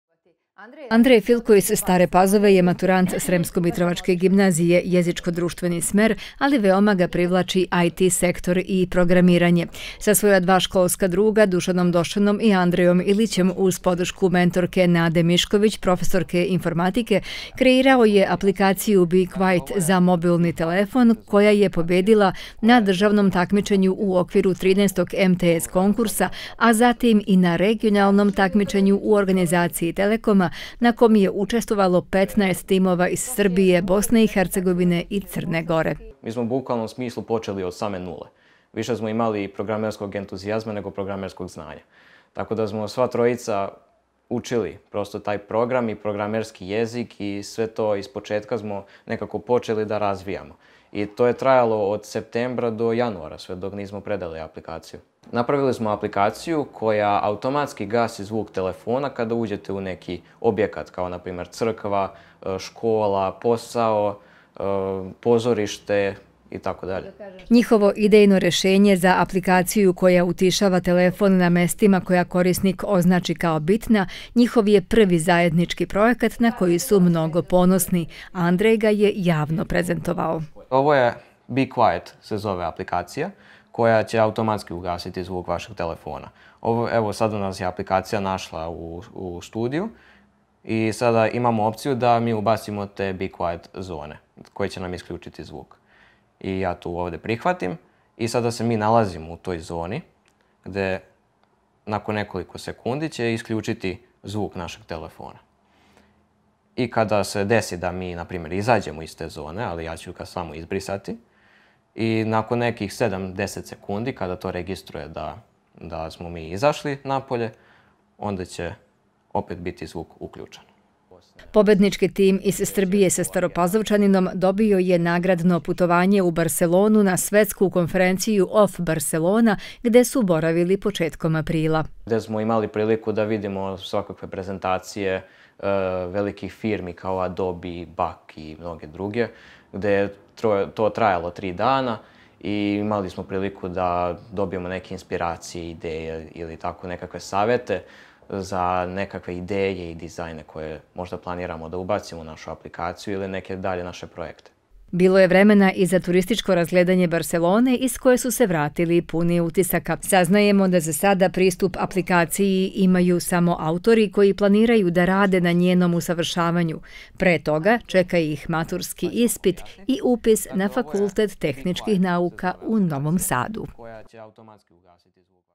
Mlad inovator Andrej Filko iz Stare Pazove
Pošto je tim učenika sremskomitrovačke gimnazije, koju pohađa i Andrej Filko iz Stare Pazove pobedio sa svojim svojim idejnim rešenjem u vidu aplikacije koja treba da olakša život na sedmom  „Regionalnom app izazovu“ koji organizuju kompanije Telekom Srbija, BiH i Crna Gora,  kao nagradu dobili su putovanje u Barselonu.  Sa ovog nagradnog putovanja vratili su se puni utisaka i novih znanja.